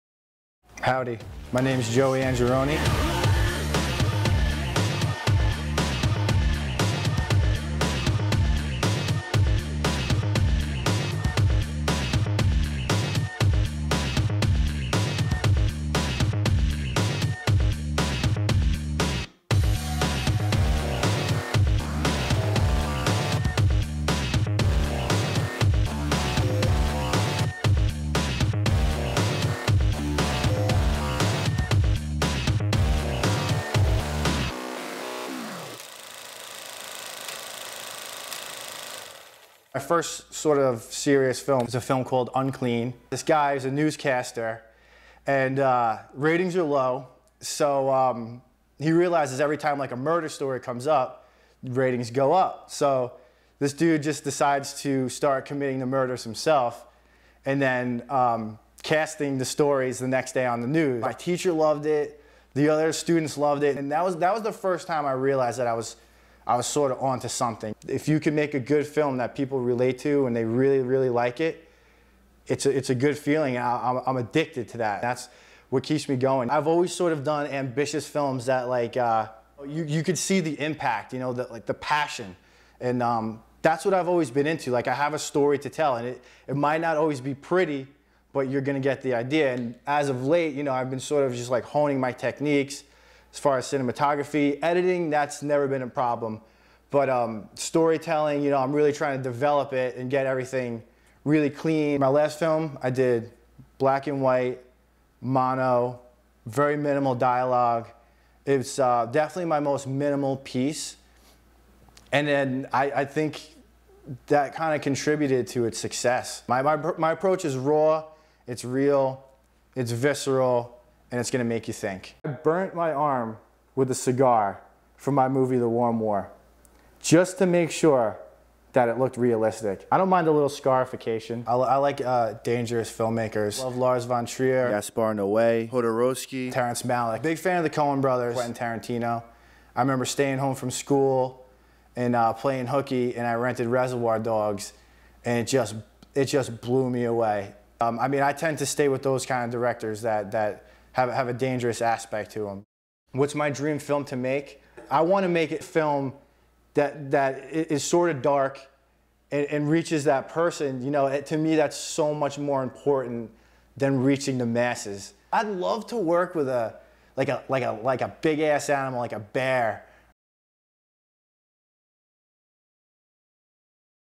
Conversations In Black: Joey Angerone
Crown Royal interviews filmmaker Joey Angerone